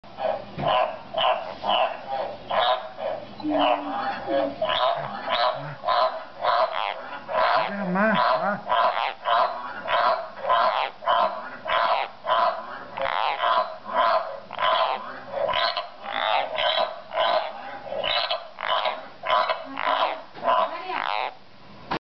Frog croaking
I was watching some frogs on youtube croaking and my frog just started to croak out of no where.